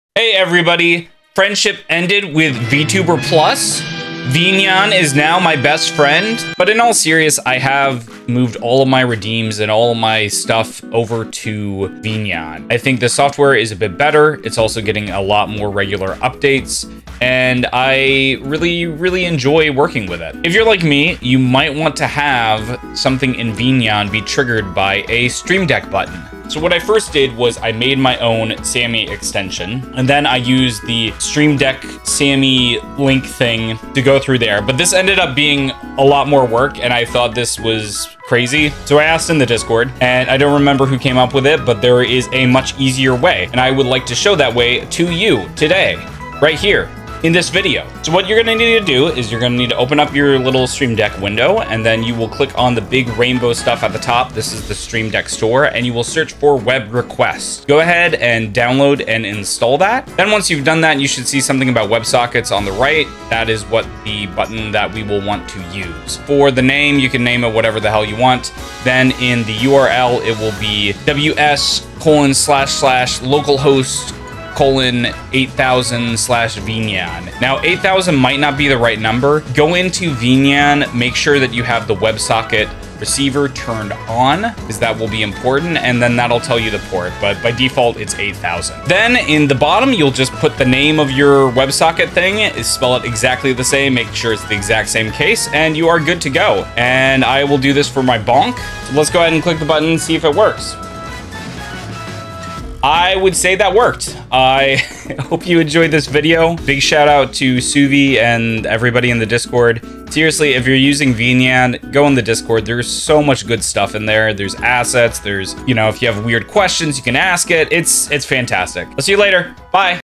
Connect VNyan to your Streamdeck
**IF YOU ARE READING THIS IN THE FUTURE. PLEASE JUST USE THE VNYAN STREAMDECK EXTENSION.

Hey everyone, I've been having a lot of fun with VNyan lately! One thing that was a bit rough early on was trying to figure out a way to connect it to my streamdeck. Thankfully there's a pretty easy way to get it going.